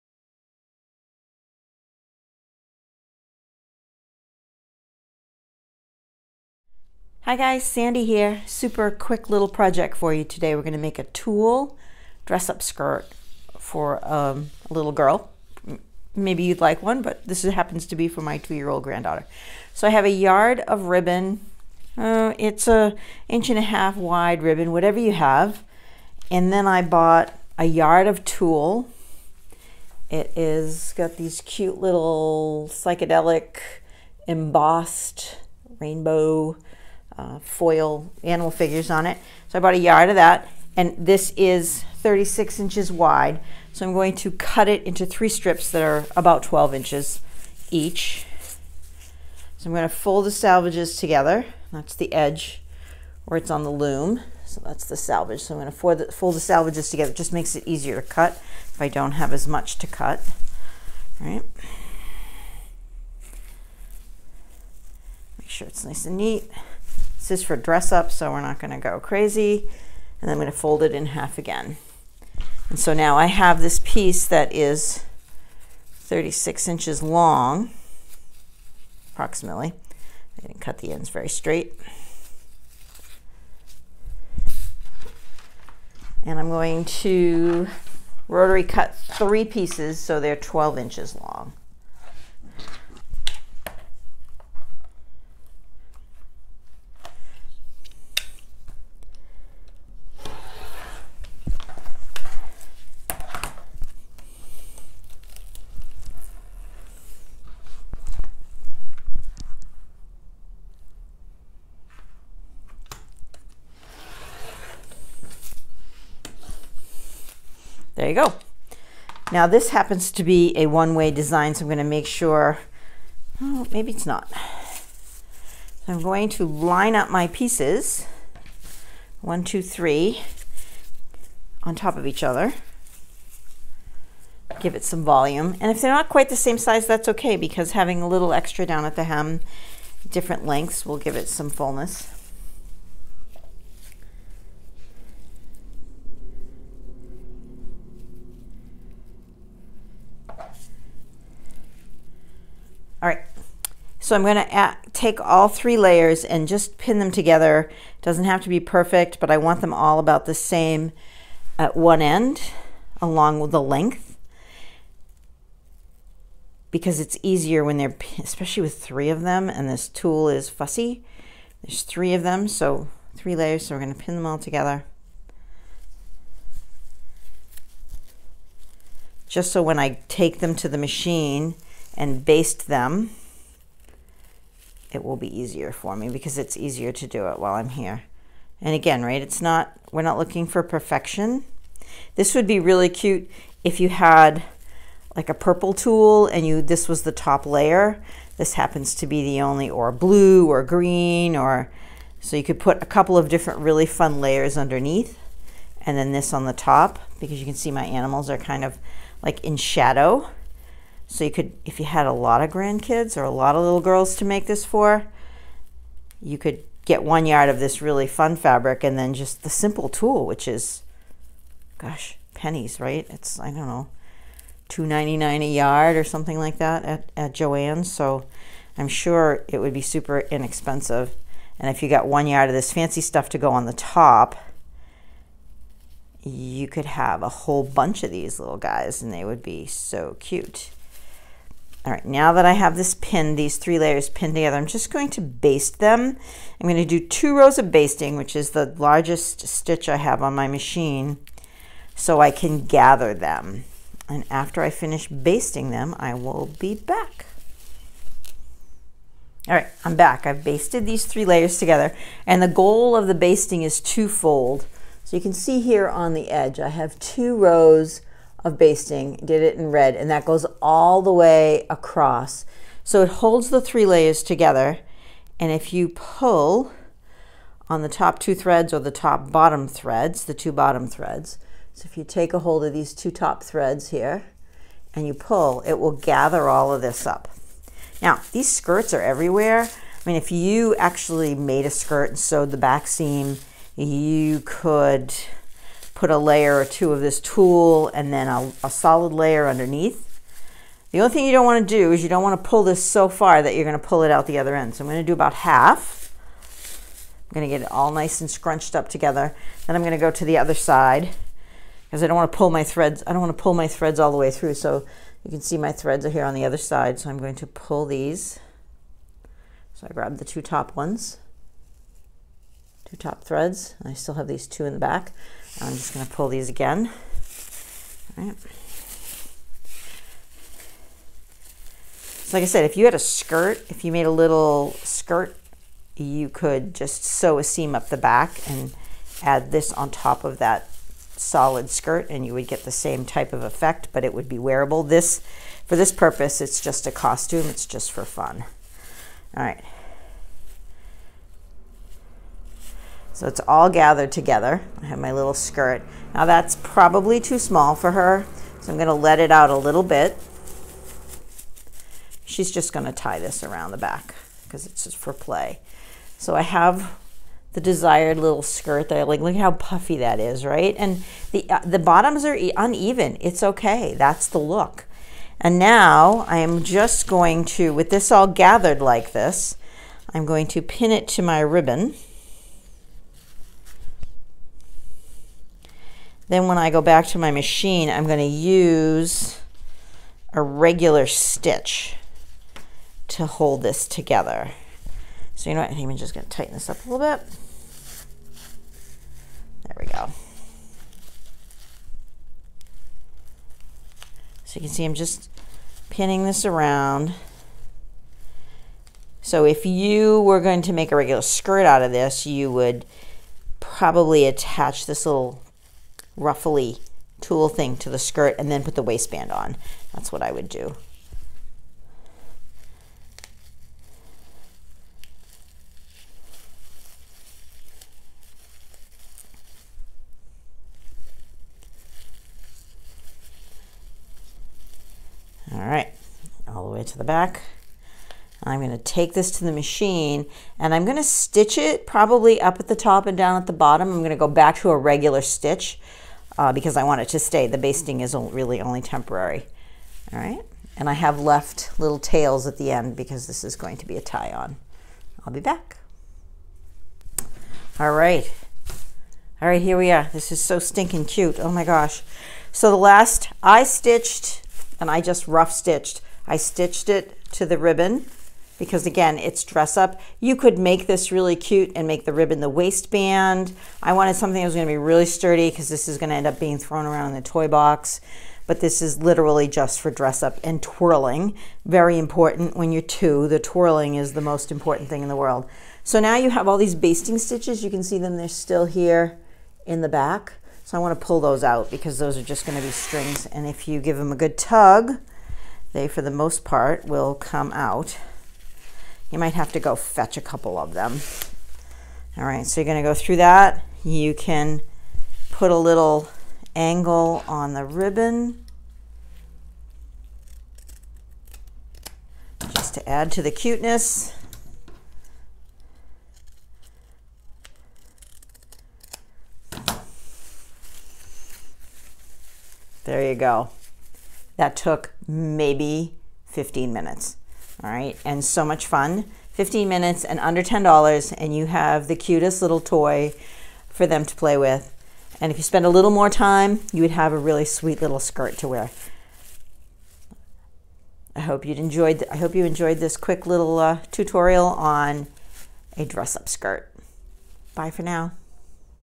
DIY Twirly Tulle Skirt
Quick tulle skirt, sew it with a yard of tulle and some cute ribbon.